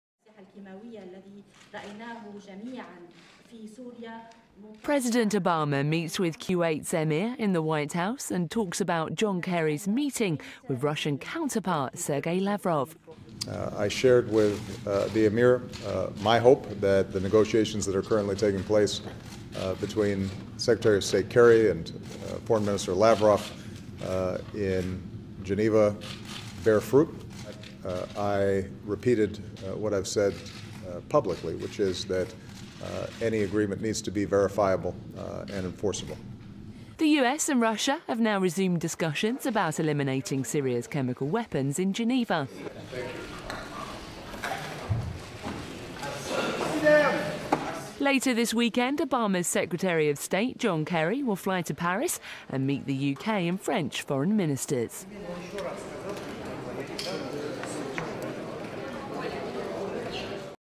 President Obama on Kerry Lavrov Syria talks Deal must be 'verifiable and enforceable'
President Obama says a deal struck between John Kerry and Sergei Lavrov on Syria's chemical weapons must be "verifiable and enforceable"